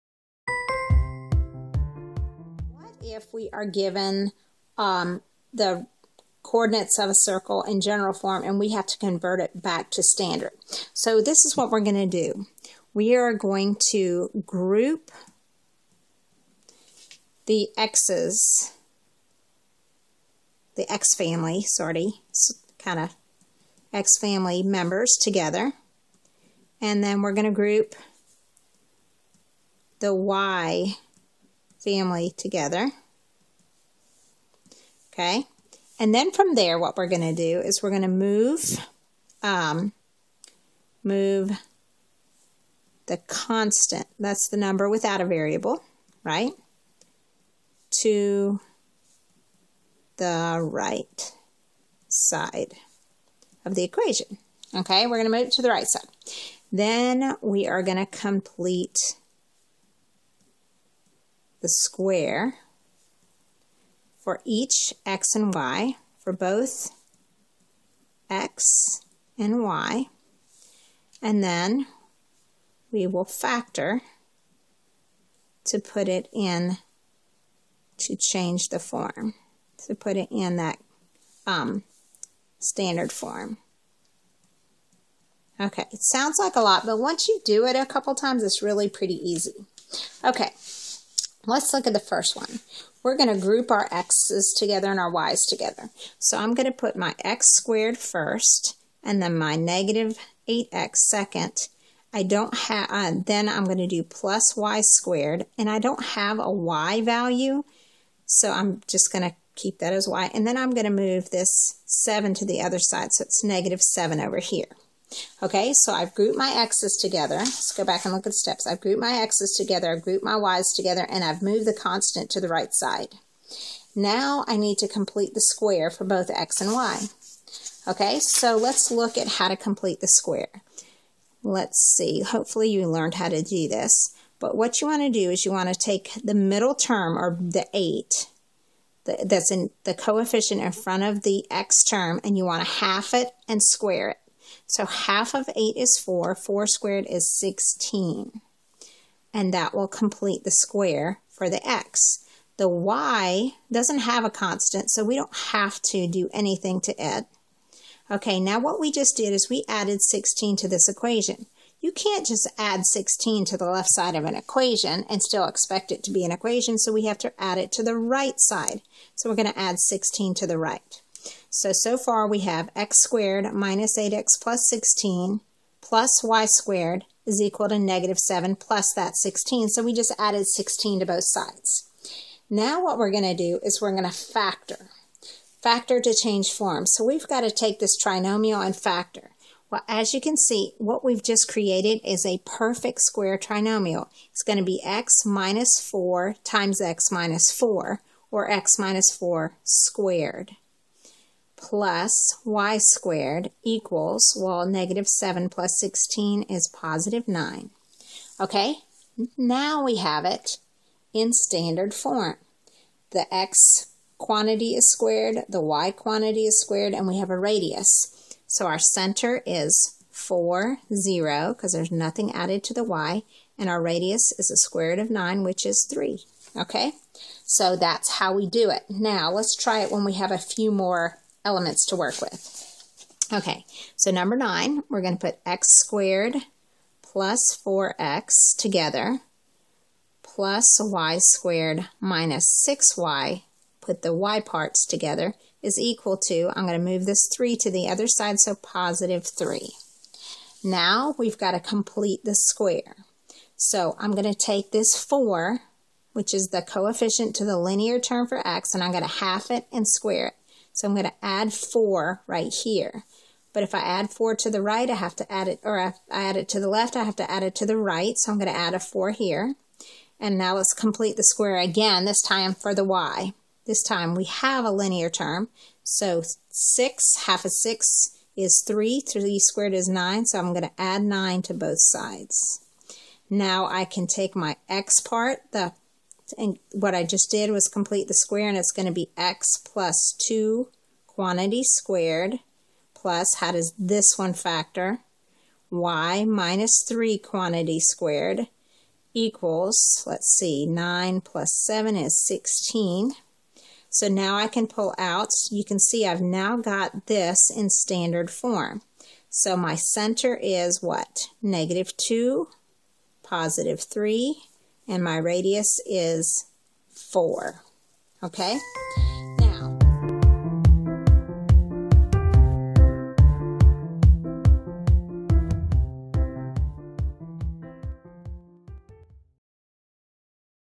Equations of Circles - Converting from GENERAL to STANDARD FORM
How to work with equations of circles. Given the coordinates of a circle, convert from general form to standard form.
Group the "x" family together
Group "y" family together
Move the constant to the right side of the equation
Complete the square for each "x" and " y"
Factor to move to standard form

Let’s connect
MooMooMath